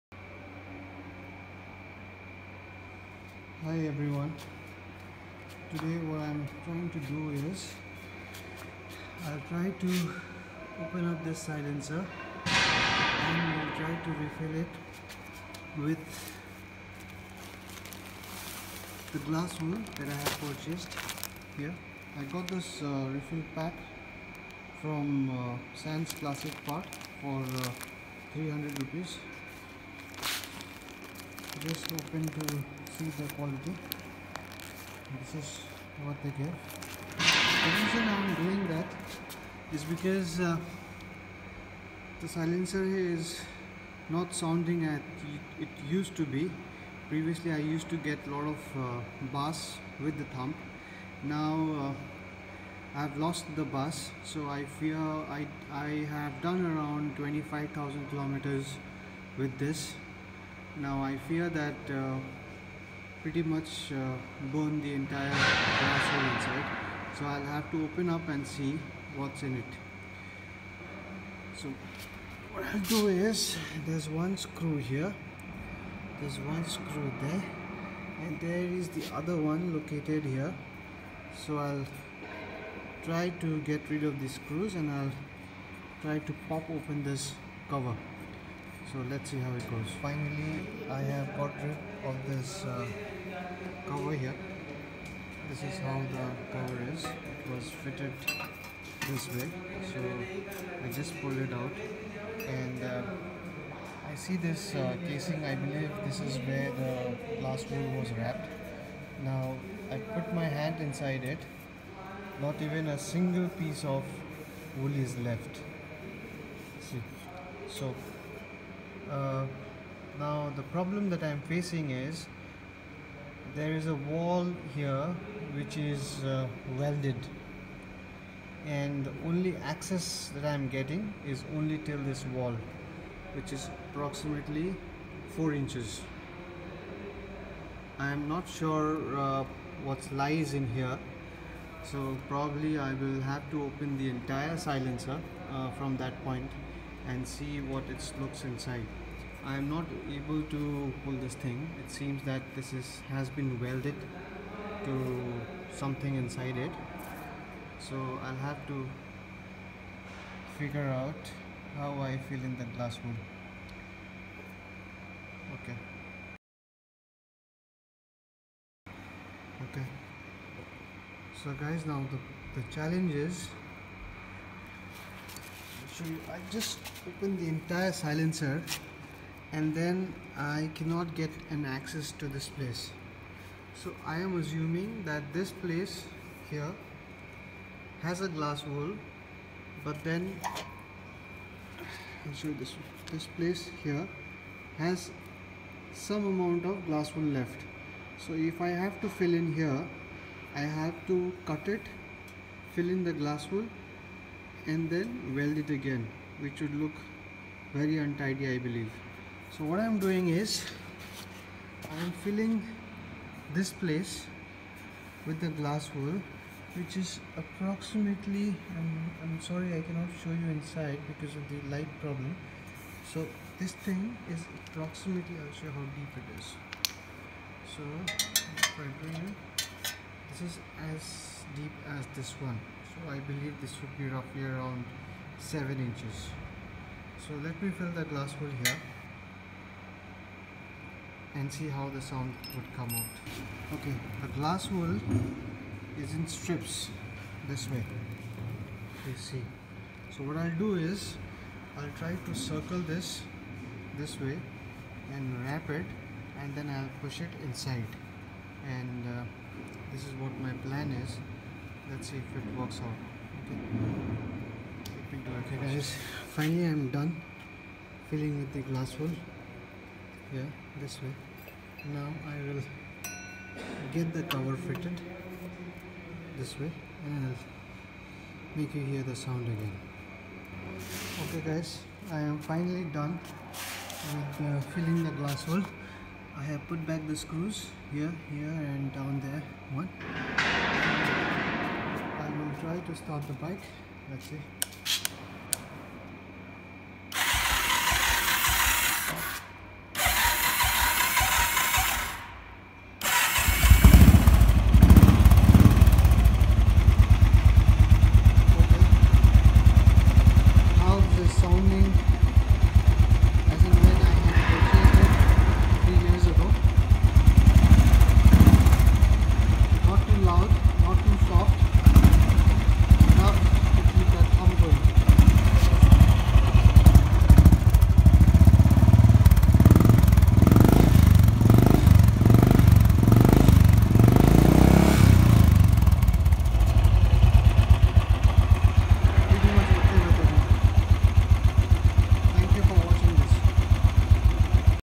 refilling glass wool#thunderbird silencer#royal Enfield #
Checking the glass wool and refilling it .. loud noise reduction,mellow thumping sound is the result,